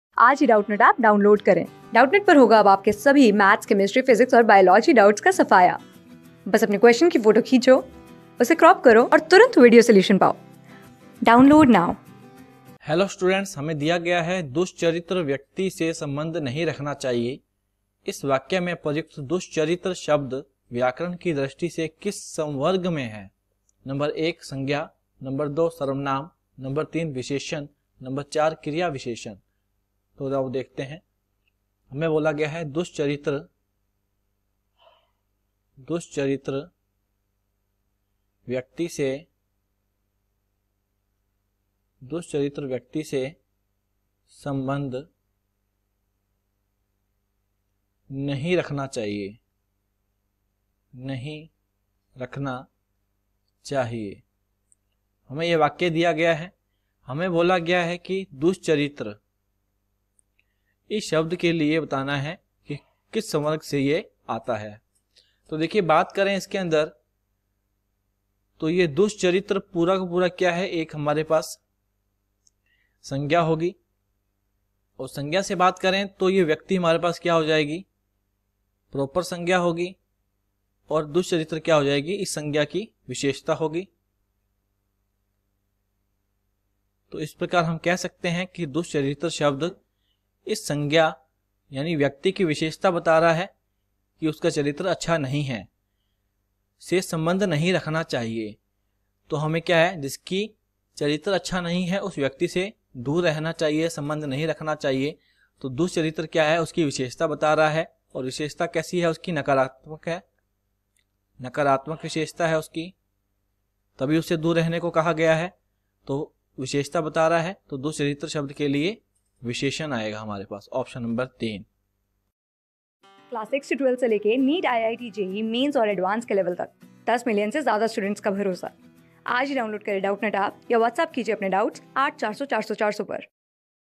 दुष्चरित्र व्यक्ति से सम्बन्ध नहीं रखना चाहिए।\' इस वाक्य में प्रयुक्त \'दुष्चरित्र\' शब्द व्याक...
दुष्चरित्र व्यक्ति से सम्बन्ध नहीं रखना चाहिए।\' इस वाक्य में प्रयुक्त \'दुष्चरित्र\' शब्द व्याकरण की दृष्टि से किस संवर्ग में हैं ?
Class: 14
Subject: HINDI
Chapter: विशेषण और विशेष्य
Board:CTET

👉 Have Any Query? Ask Us.